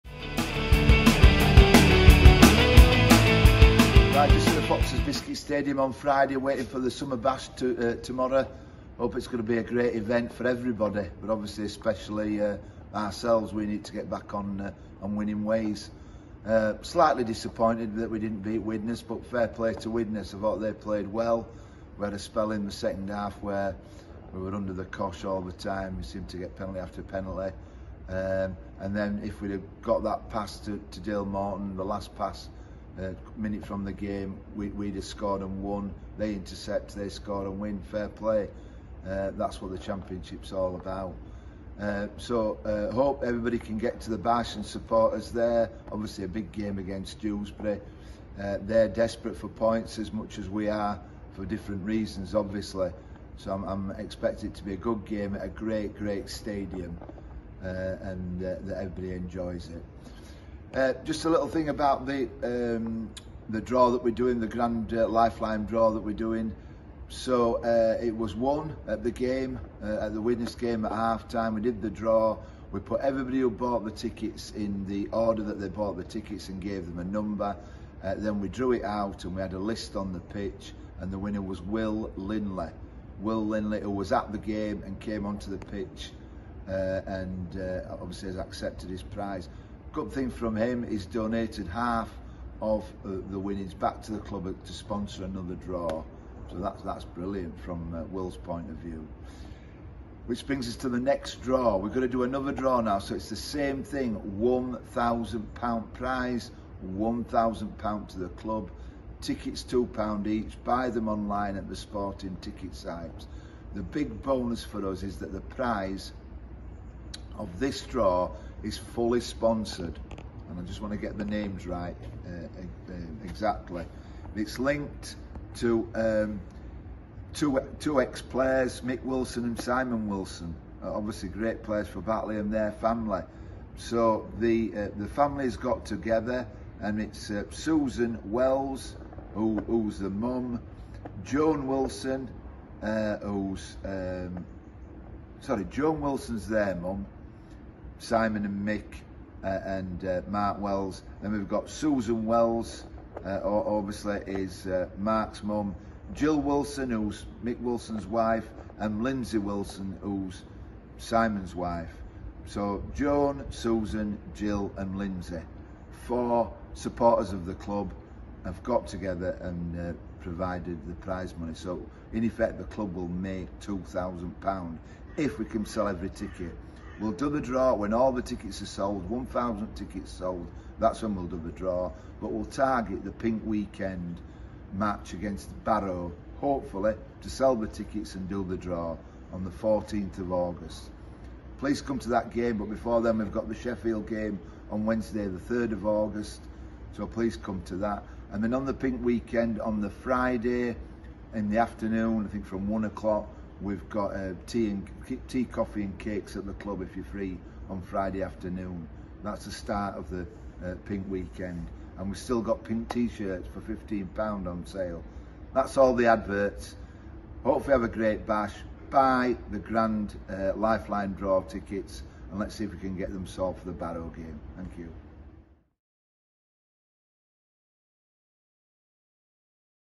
Summer Bash Weekend
Kevin's update ahead of this weekend's Summer Bash